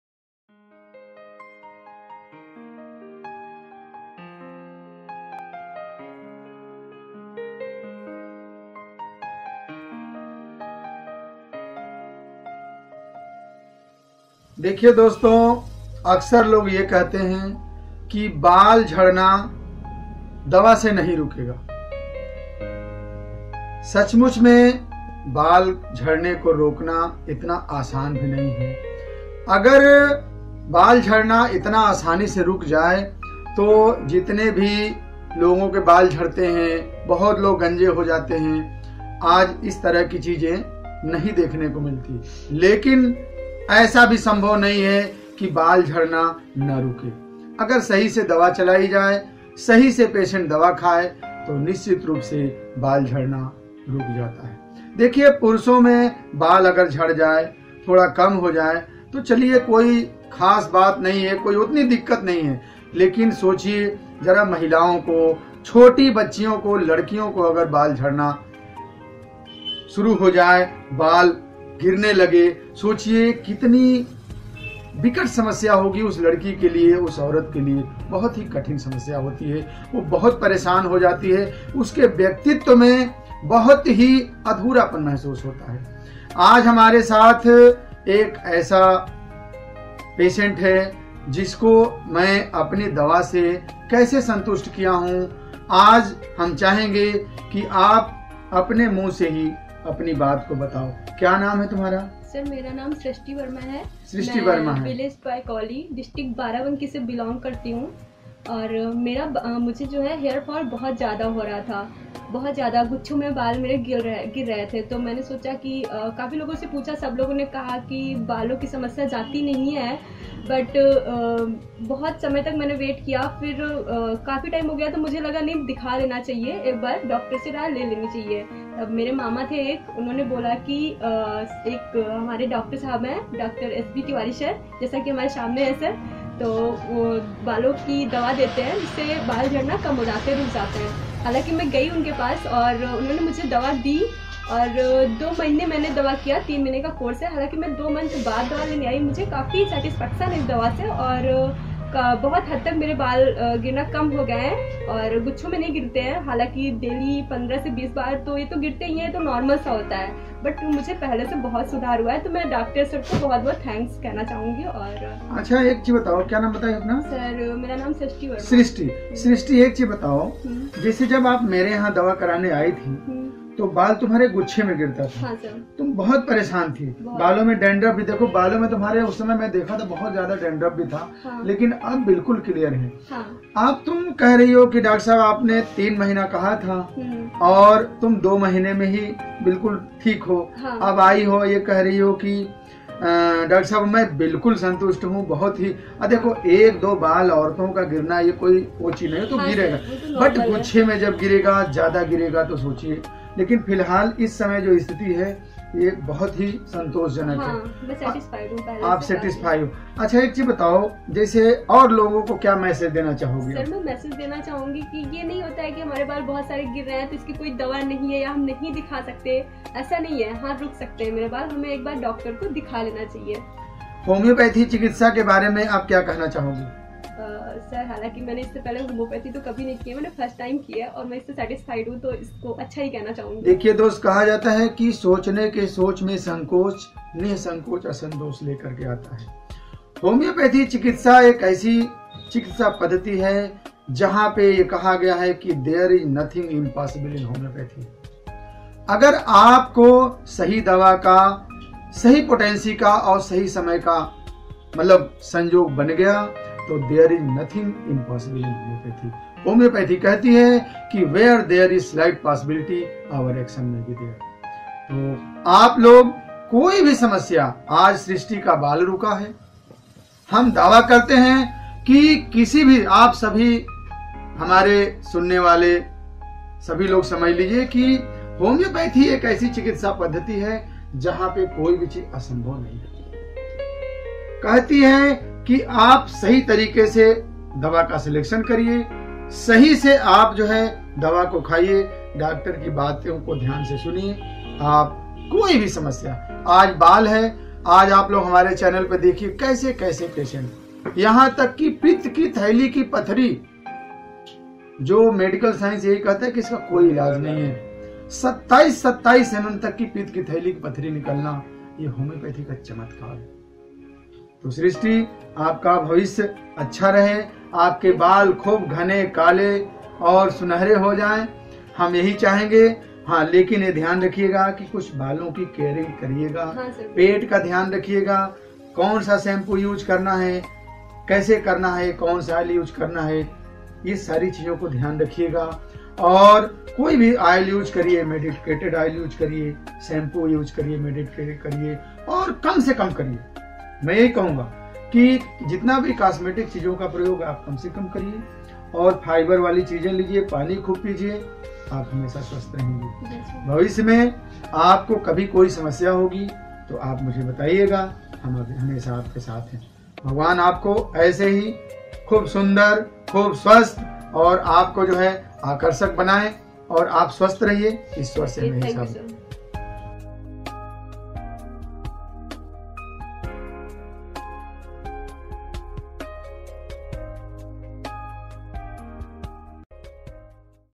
HAIR FALL TREATMENT#ALOPESIA# सृष्टि की कहानी, DR.S.B.TIWARI# KALYAN JI#LIMCA BOOK RECORDER#LUCKNOW
FALLING OF HAIR
WEAKNESS OF HAIR
HAIR LOSS
ALOPECIA AREATA
प्यारे भाइयों/बहनों
आज की कहानी उस शख्स की है जो एक ऐसी समस्या जो वाकई पुरुषों के लिए तो फिर भी गनीमत है मगर स्त्रीयों के लिए तो अत्यन्त ही गम्भीर है जो आजकल अक्सर देखने को मिलता है। हम बात करेंगे Alopecia बालों के झड़ने की। क्या कहना है बाराबंकी की सृष्टि का आप खुद देखिए.